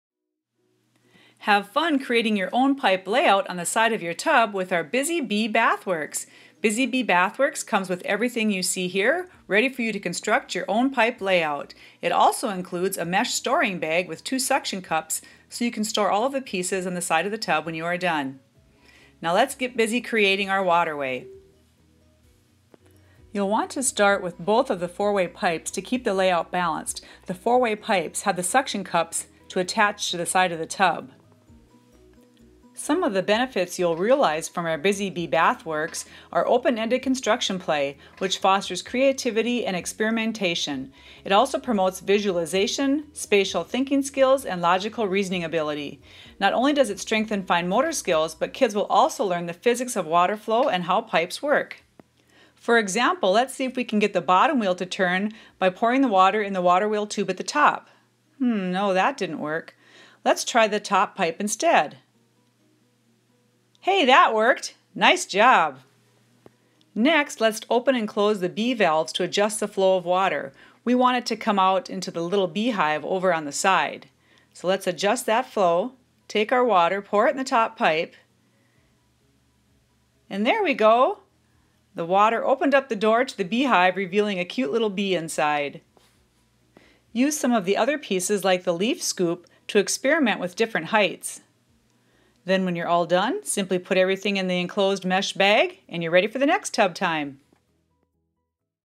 Discovery Toys - Busy Bee Bath Works
#2775  BUSY BEE BATH WORKS

Have fun creating your own pipe layout on the side of your tub.  Open & close the bee valves to adjust the flow of water.  Experiment with different layouts to direct the water to the desired locations.  Store the components in the included mesh bag (with suction cups for hanging) to dry.  Includes a Waterwheel Tube with Holder, a Flip-Open Beehive, a Sunwheel, two 4-Way Pipes, four Bee Valves, four Straight Pipes, four L-Pipes, a Leaf Scoop, and a Mesh Storage Bag.

from 3 years+
dt exclusive set